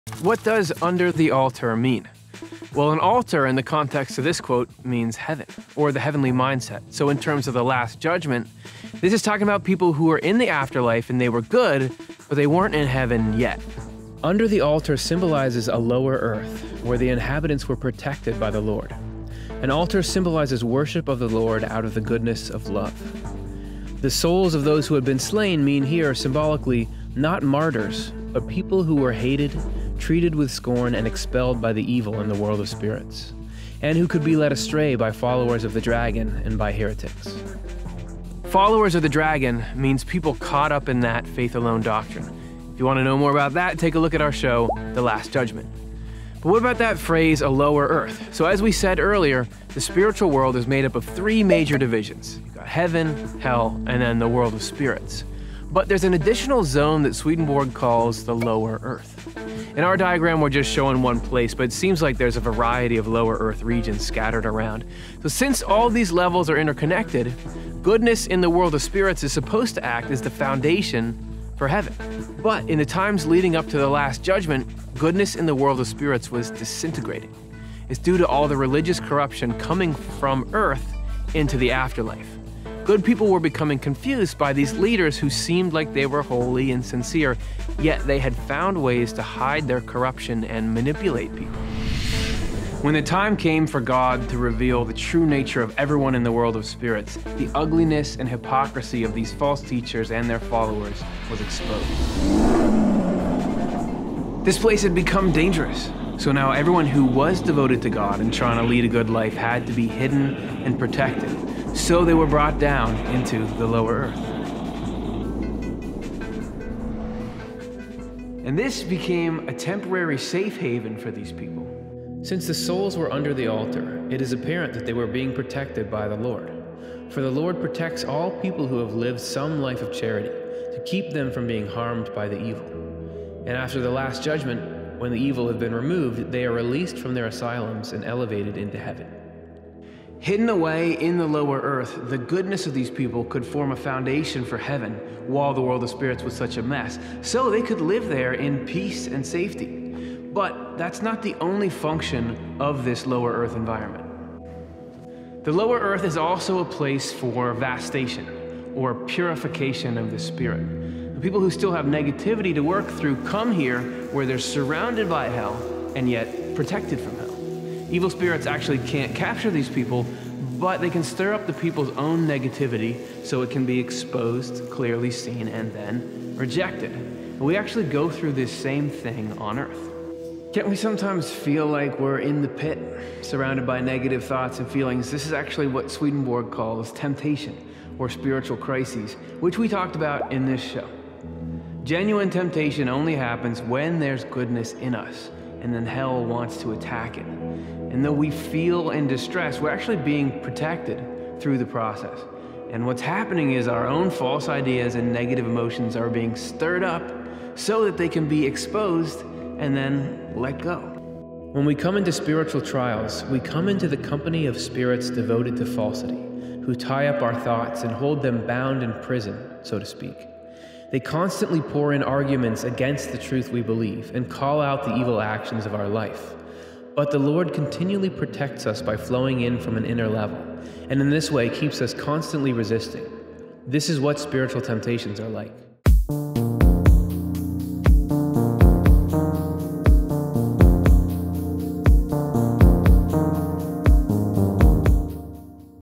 Who Are the Souls Under the Altar? - S&L Short Clips
Swedenborg witnessed the Last Judgment in the spiritual world, and says this process also happens in our minds. Learn the meaning of the symbolic imagery in Revelation 6:9.

In the full-length episode "The Meaning of the Book of Revelation: Souls Under the Altar," we discuss Emanuel Swedenborg’s writings about the hidden meaning of the souls under the altar.